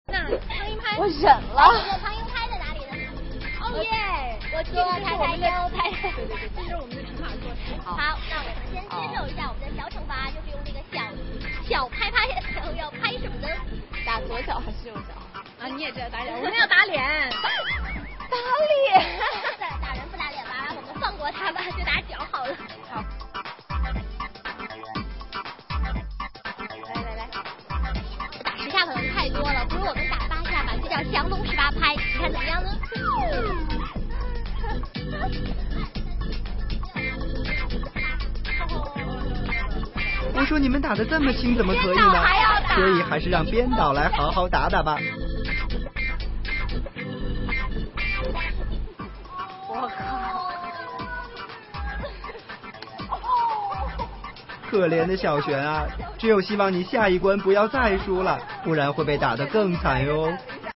ayawawa punish her partner
ayawawa discuss with other emcee on how to punish their companions